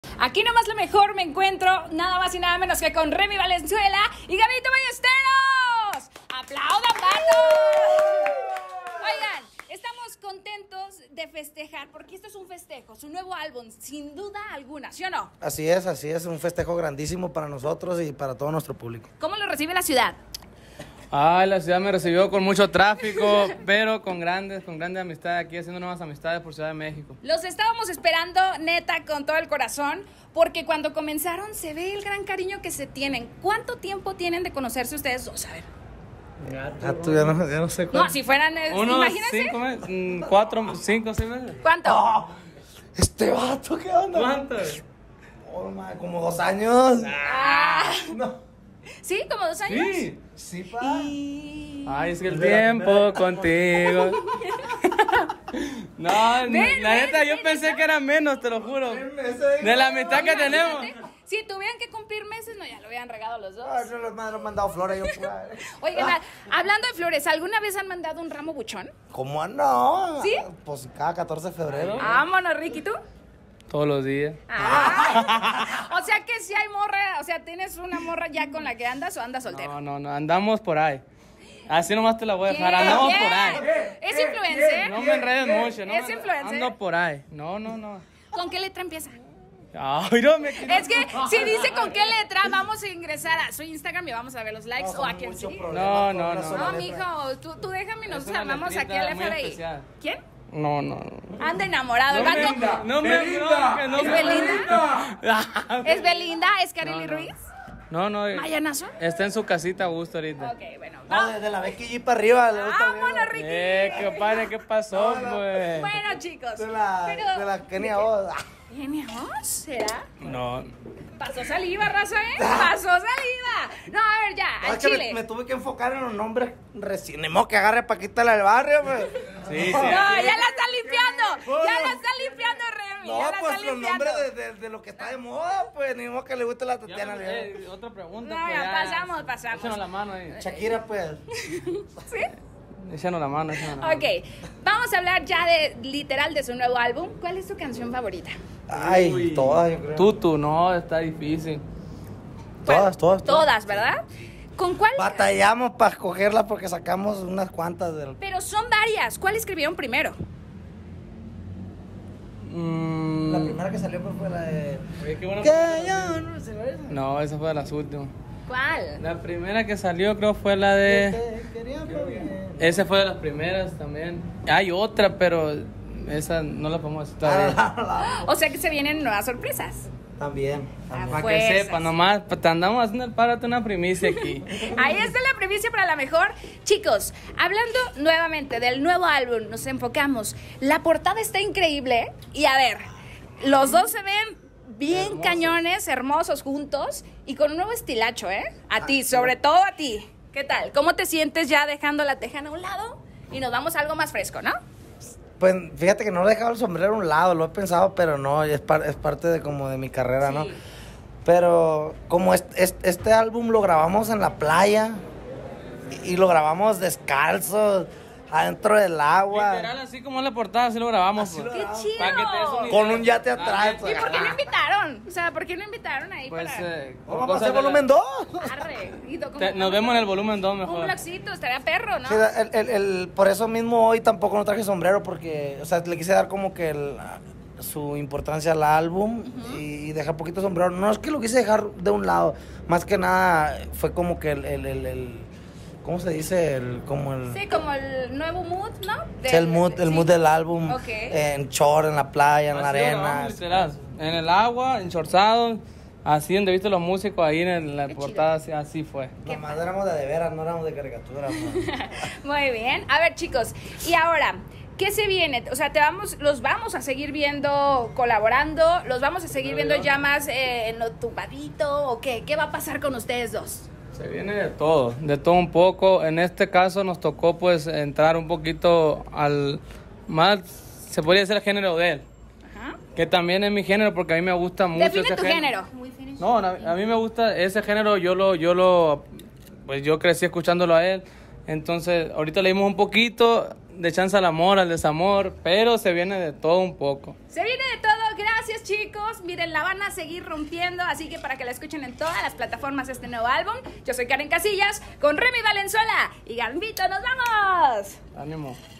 Remmy Valenzuela y Gabito Ballesteros llegan con un álbum lleno de talento
En exclusiva para La Mejor 977, Gabito Ballesteros y Remmy Valenzuela revelaron las grandes sorpresas que se viene junto a su más reciente álbum "SS 23" ¿Qué tanto dijeron?